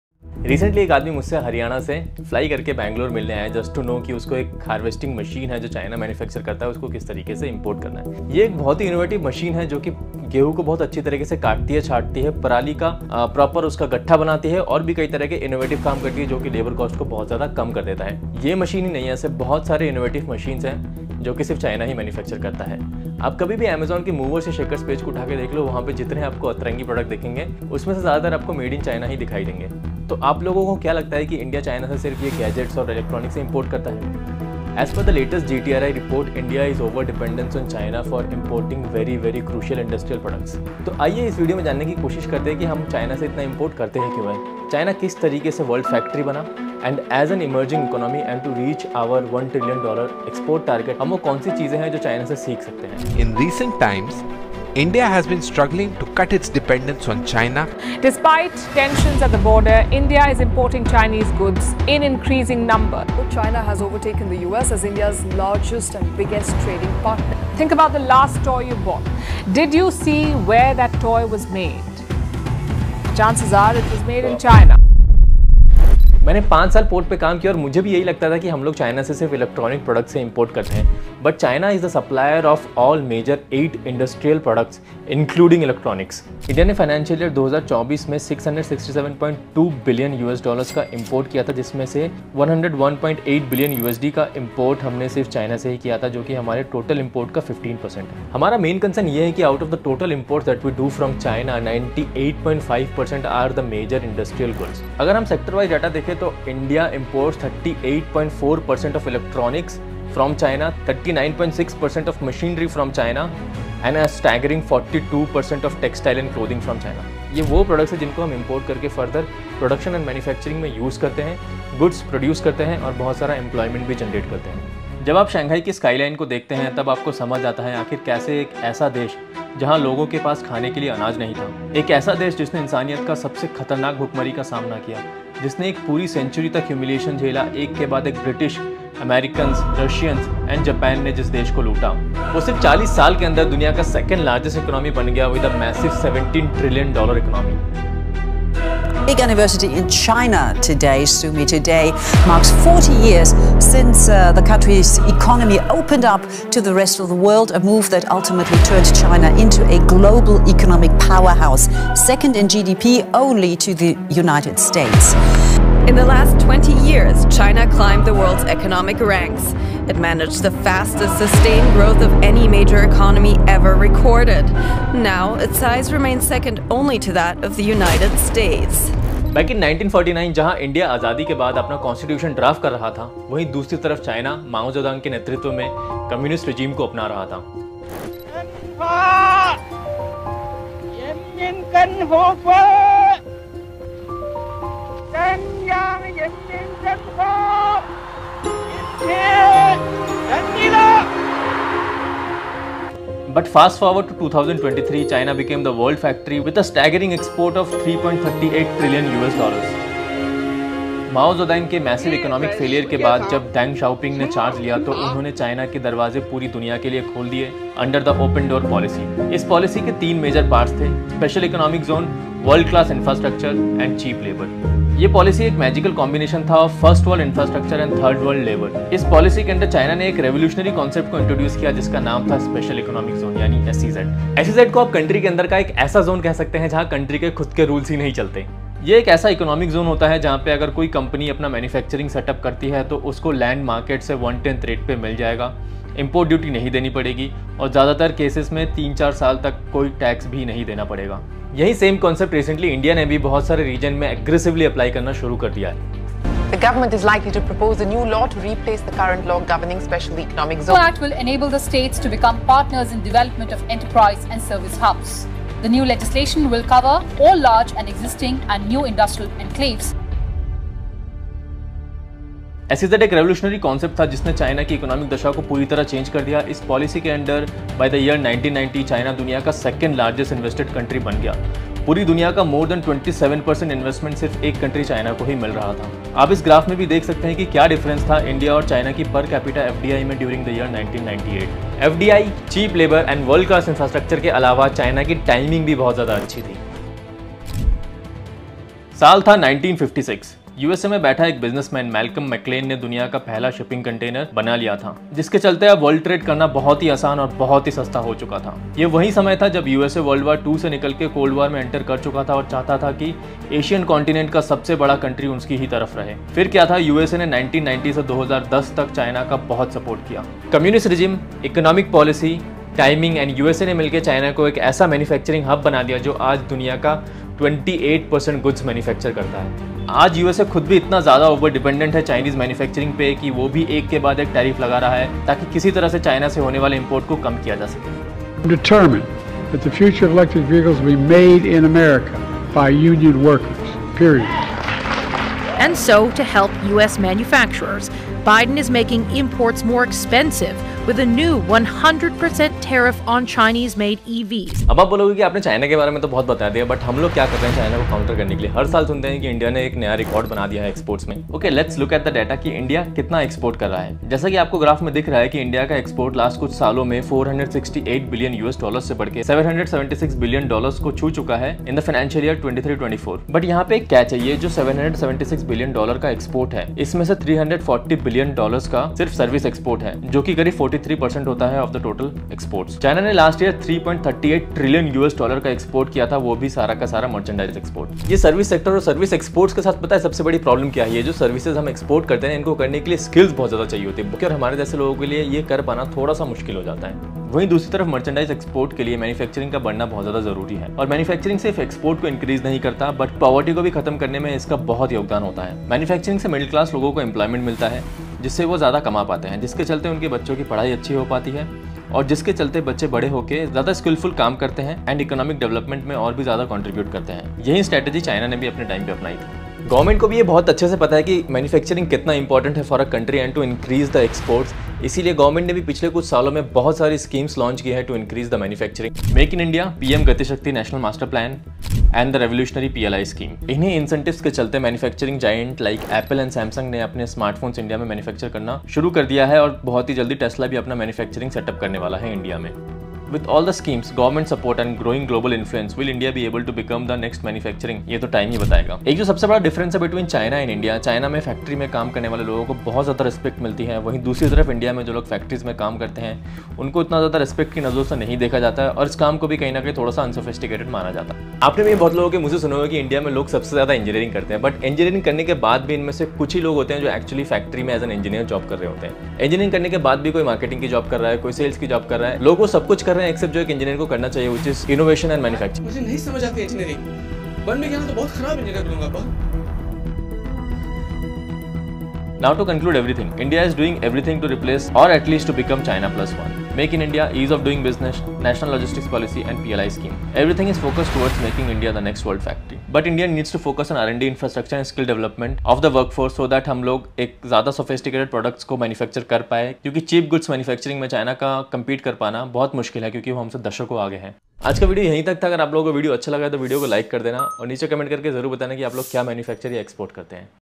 How China Became The World Factory To Exports $3.38 Trillion | Why India Is Over depended On China?
Hello Everybody, welcome to the only channel in India where you will find technical, data-backed, actionable import export and business videos

India's Manufacturing Revolution: Can We Become the Next China?

In this video, we dive into India's growing manufacturing sector and explore its potential to become the next global manufacturing hub. We'll examine the factors that have contributed to China's dominance in manufacturing, analyze India's current position, and discuss the government initiatives aimed at boosting domestic manufacturing.
Key Points:
China's success as a manufacturing hub can be attributed to its open-door policy, world-class infrastructure, cheap labor, and strategic timing.

India's export growth has been impressive, but a significant portion is service-based. Manufacturing remains crucial for poverty reduction and economic development.

Government initiatives like Make in India, PM Gati Shakti National Master Plan, and the PLI scheme aim to attract manufacturing giants to India.

India needs to focus on R&D, infrastructure, and workforce skill development to produce sophisticated products and compete effectively.

✅ For personal consultation with me click the link below (Paid consultation) ✅